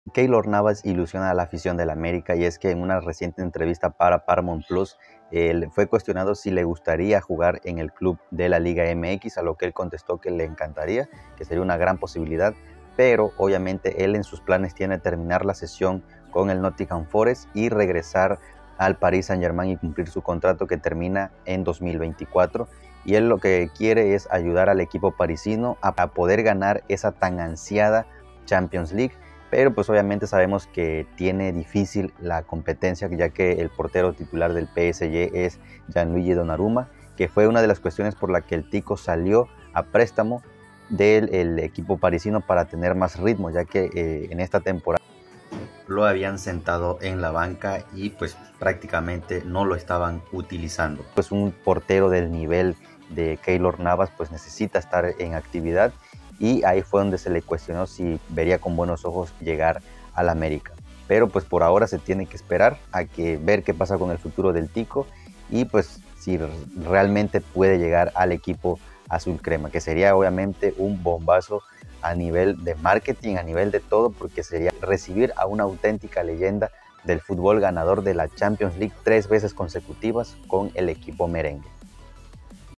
Keylor Navas llegaría al América? Fantasía o Realidad
Keylor Navas llegaría al América? Fantasía o Realidad , en un entrevista que dio el tico se dio el rumor que ilusiona a la afición azulcrema.